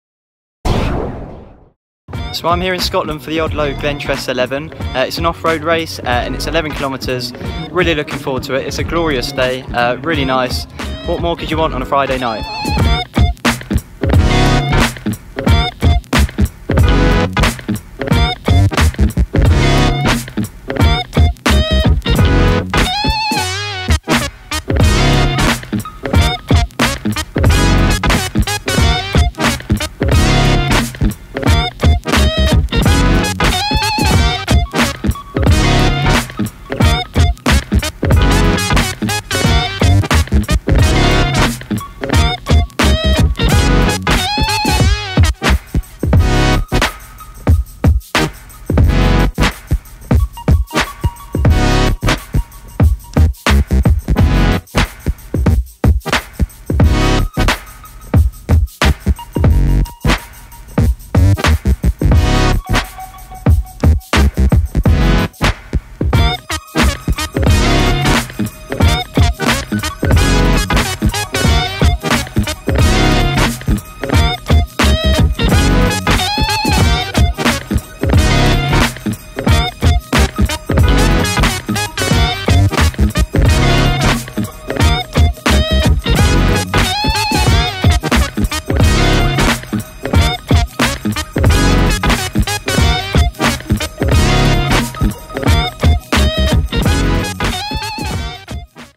The climb: Odlo Glentress 11
Josh Puttock travels to Scotland for the scenic Odlo Glentress 11; a testing 11K race around Glentress Forest. Check it out!